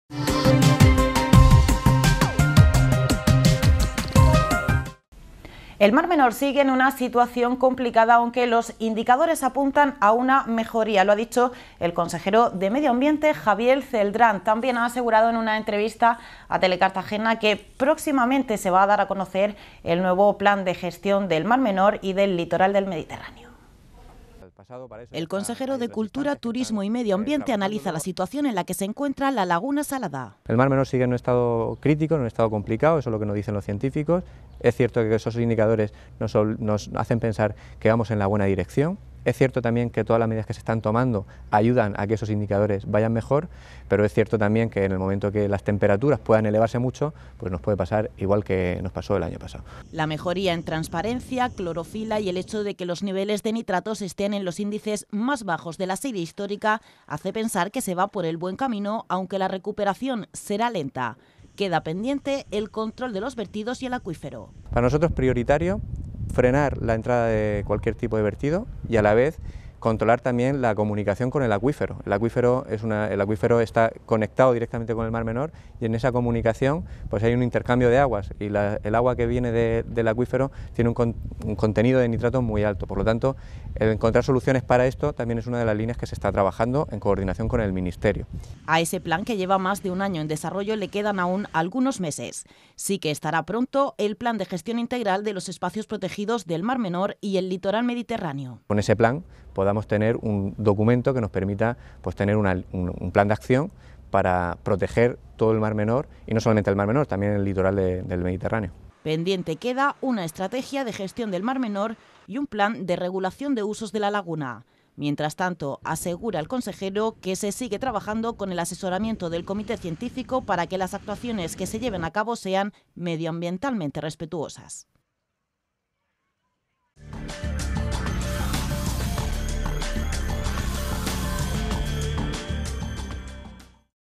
Mar Menor
El Estado del Mar Menor sigue siendo crítico pese a la mejoría de los indicadores. La Comunidad tiene casi listo el plan de gestión integral.